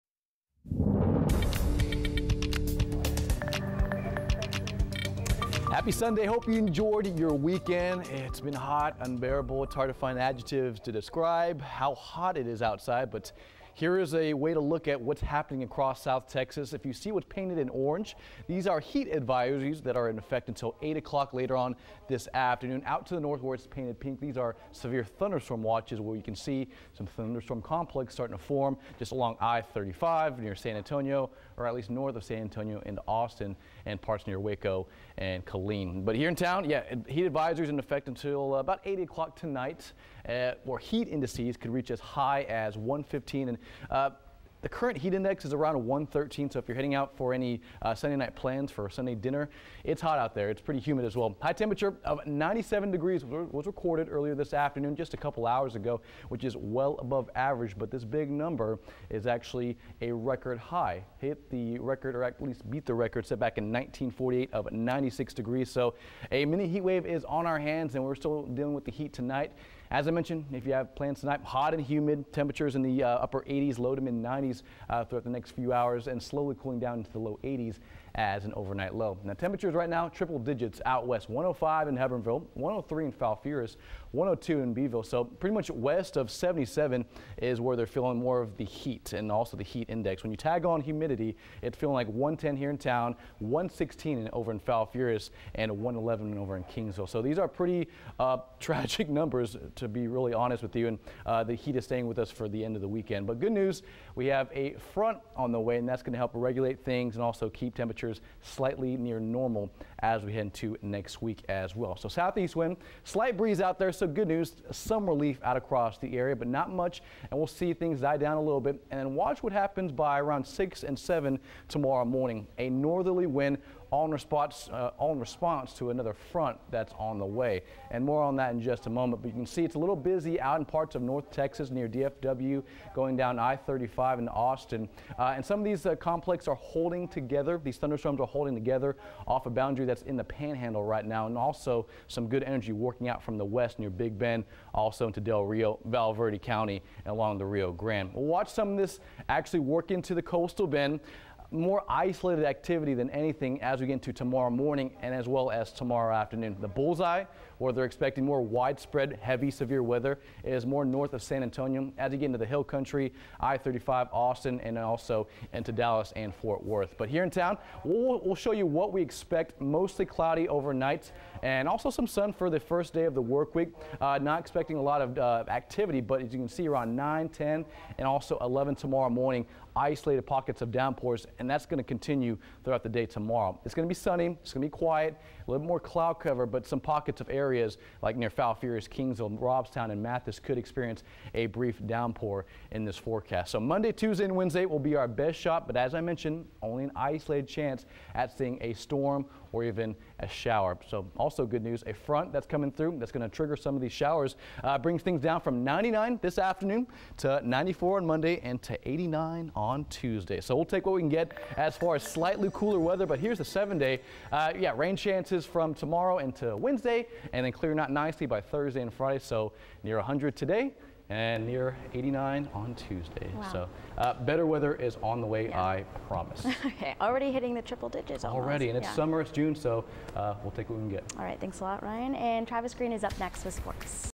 Ryan Shoptaugh's South Texas Forecast: Weak Cold Front; Isolated Showers
Isolated showers through Wednesday.